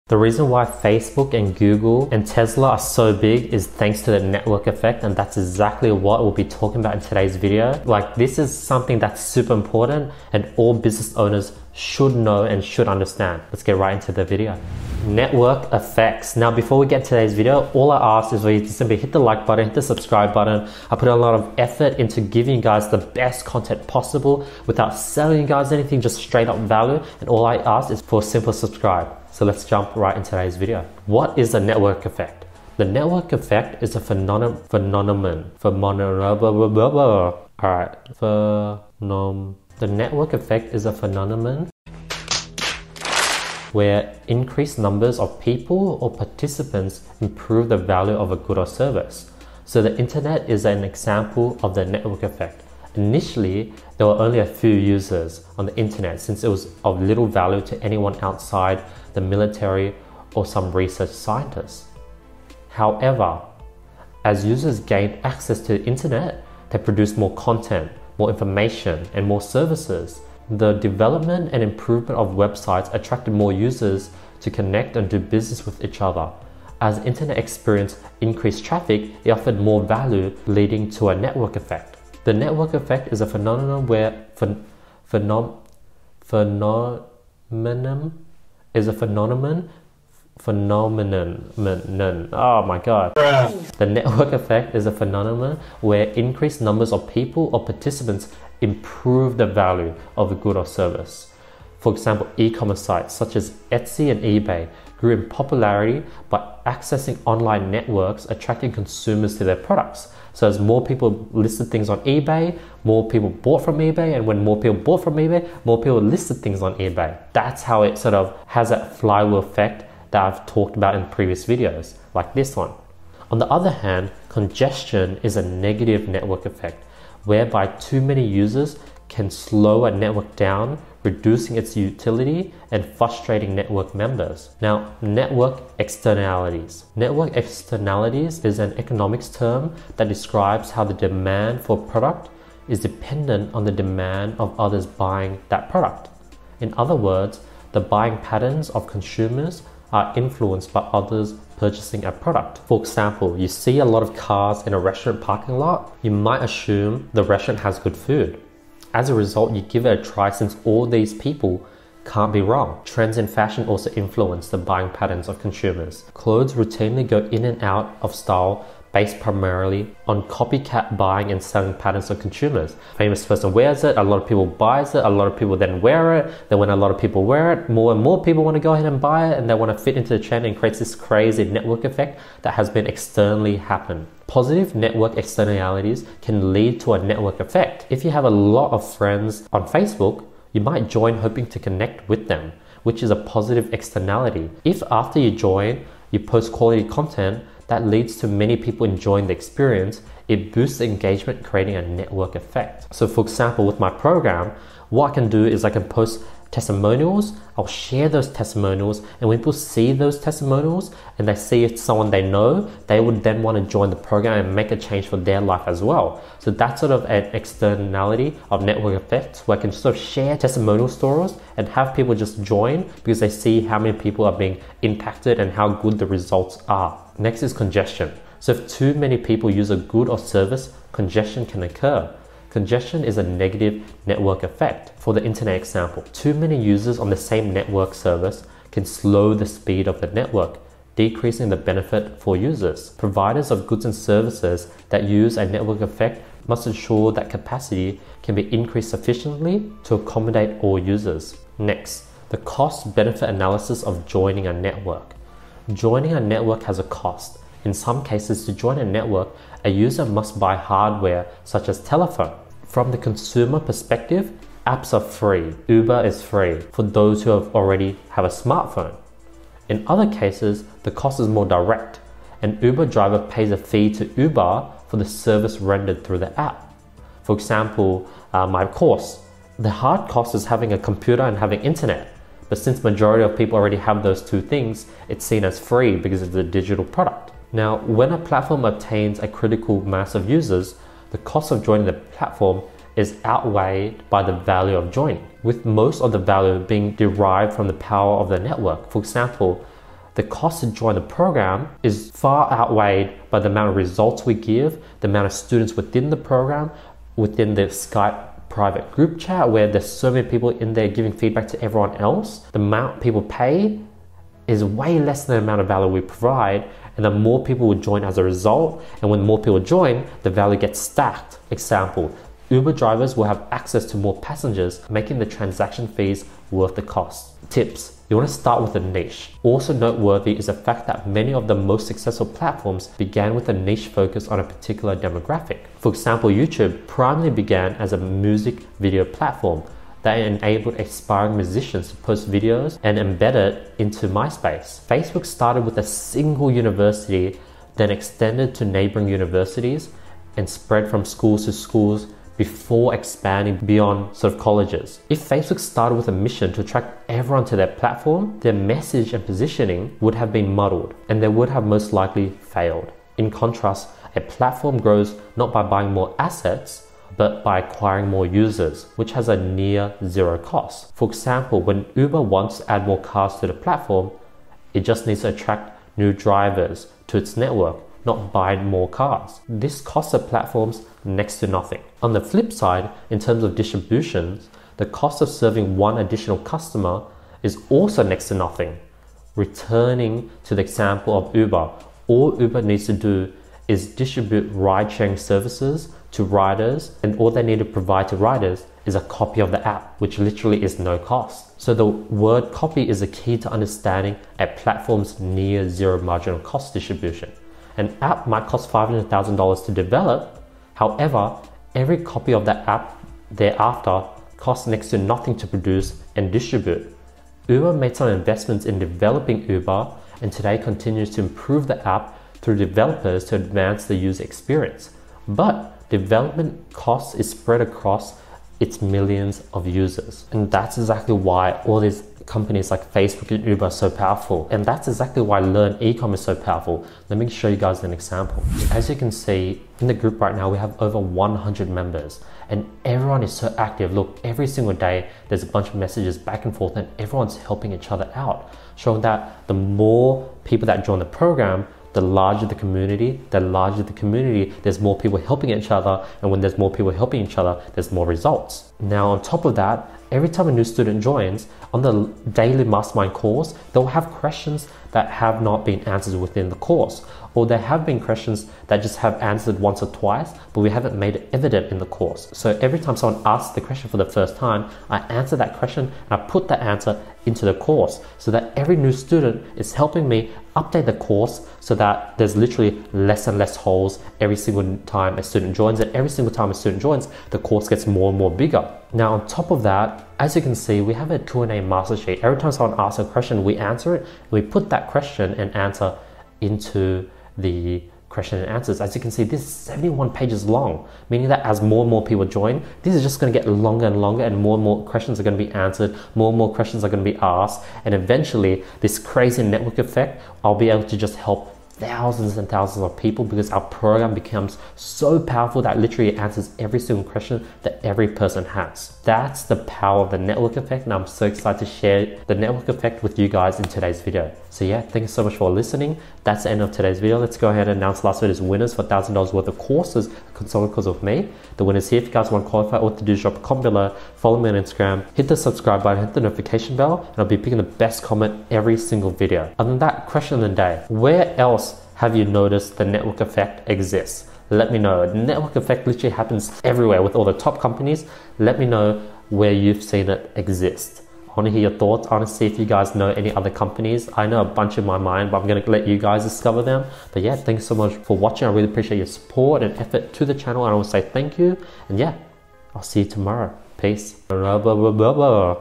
What Is The "Network Effect" In Business?
The Network Effect is described as an additional user of goods or services which has on the value of that product to others. When a network effect is present, the value of a product or service increases according to the number of others using it.

Essentially, it's improving a good or service as more and more people use it.

E-commerce websites can create a network effect as more people use it to create their stores and sell their products. The service and value increases with each user.

📚 This video's slides are available in the resources below :)

Answering ALL Questions, Mentorship Requests, Video Requests, Help
- @andyqmai (instagram)